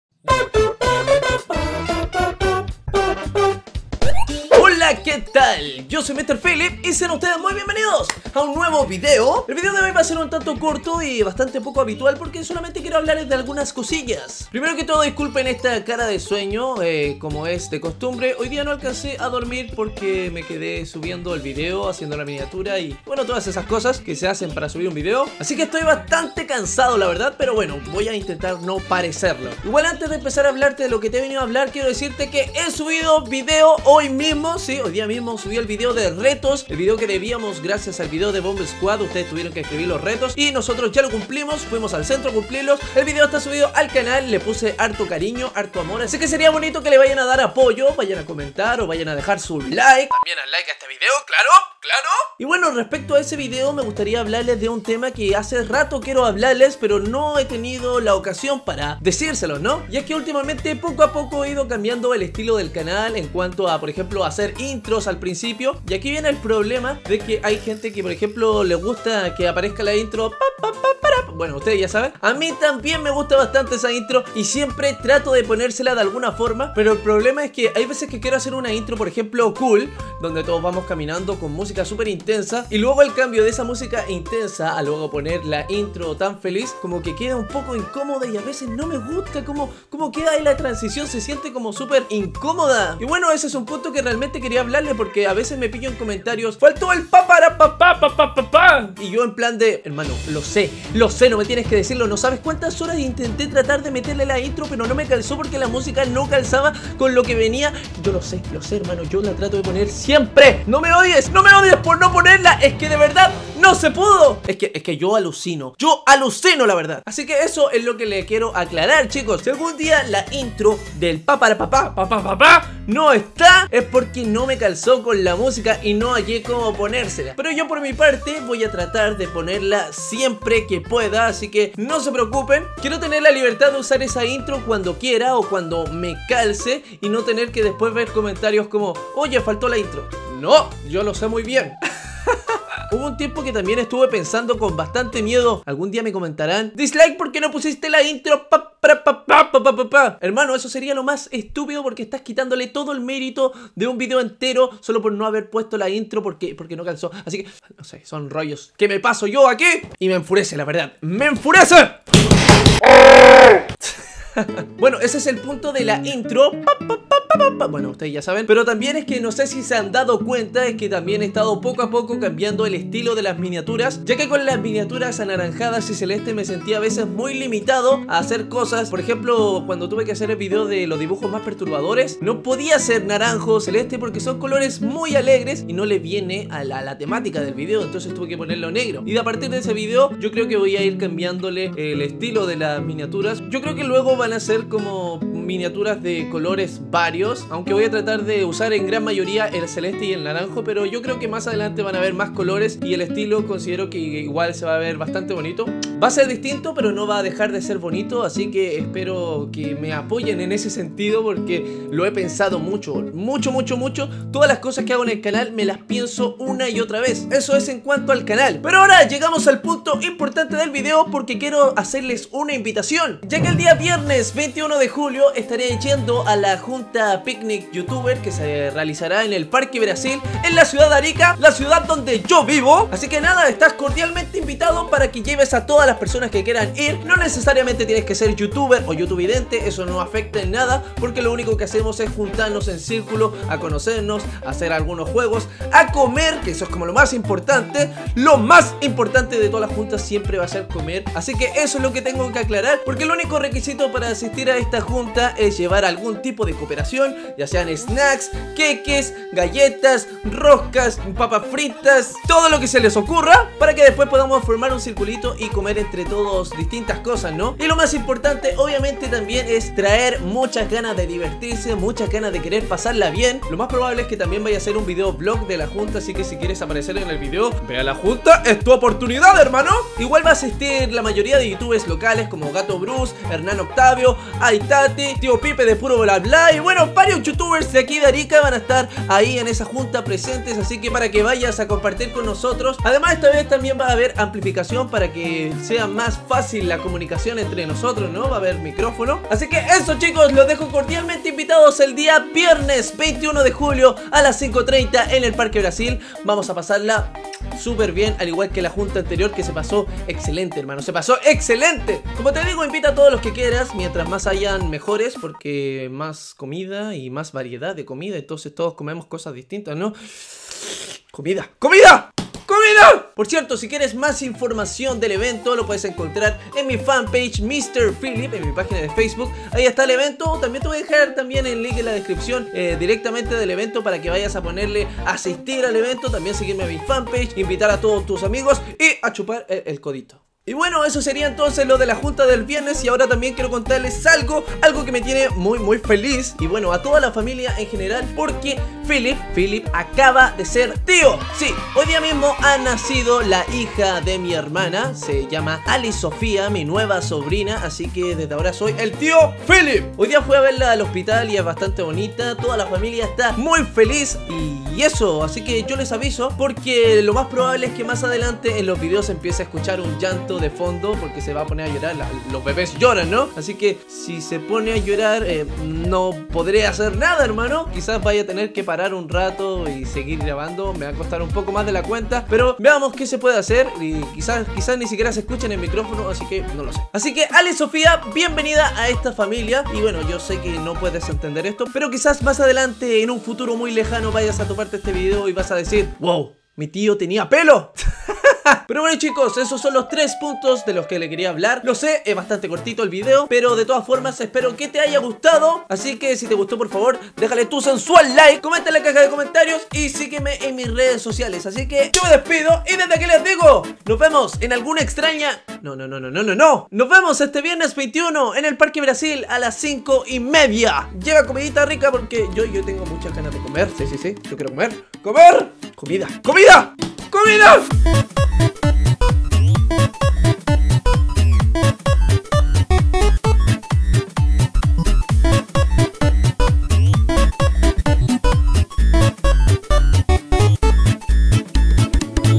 INVITACIÓN A JUNTA PICNIC YOUTUBER 2 - MR. PHILLIP
En este video hablaré varias cosas, entre ellas algunas aclaraciones y cambios del canal, pero la más importante es invitarlos a la junta que se realizará este viernes 21 DE JULIO.

 -   D  I  B  U  J  O  S  -

APOYAME CON UNA DONACIÓN PARA INVERTIR EN EL CANAL :D